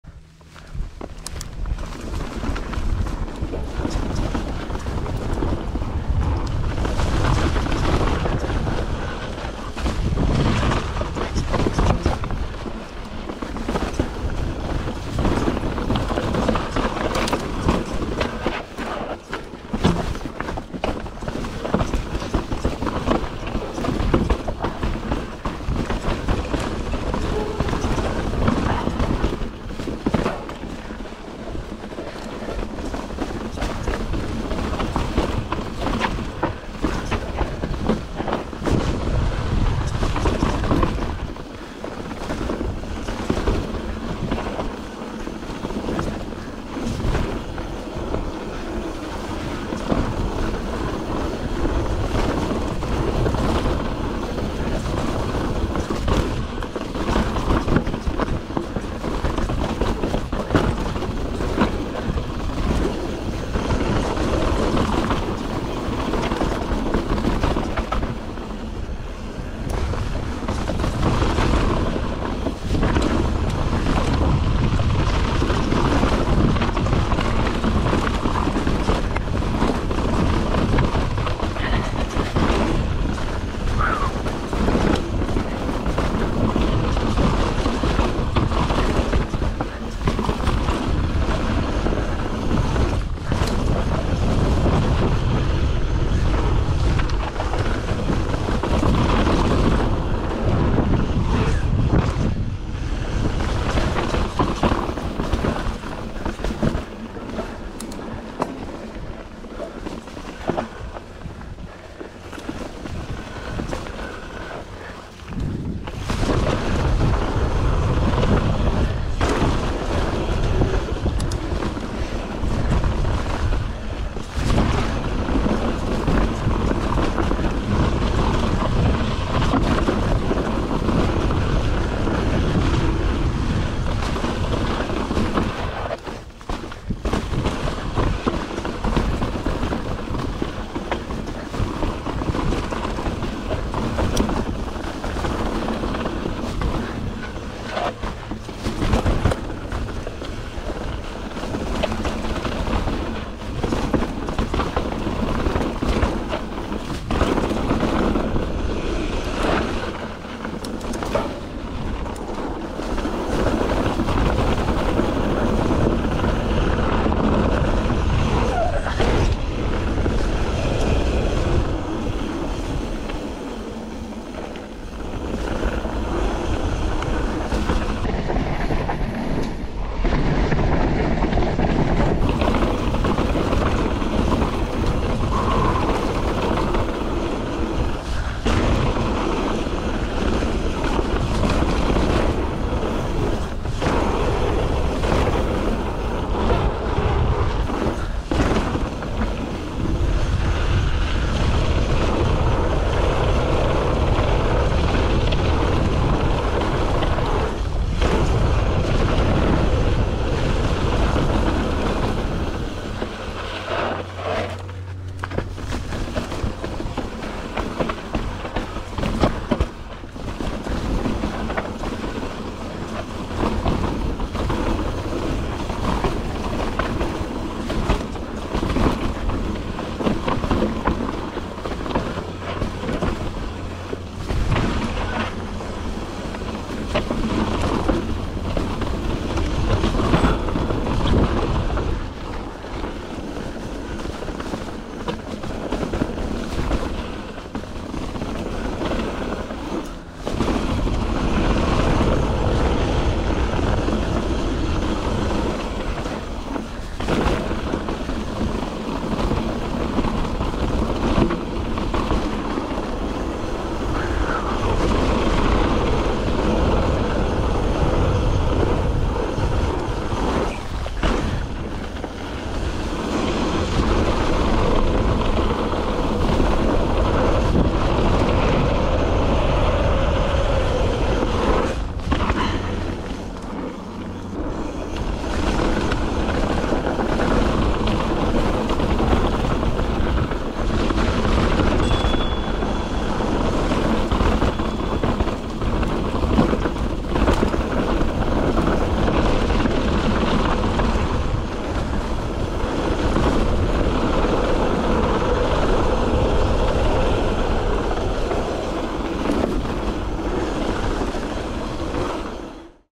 SUICIDE TRAIL | Agoura Hills, CA | YT Jeffsy | MTB mountain biking
Riding a bucket list trail for the first time. This is the Suicide Trail in Agoura Hills, CA. Park at Oak Canyon Community Park, and ride up Dead Cow Trail. From their, drop into Suicide and hold on tight.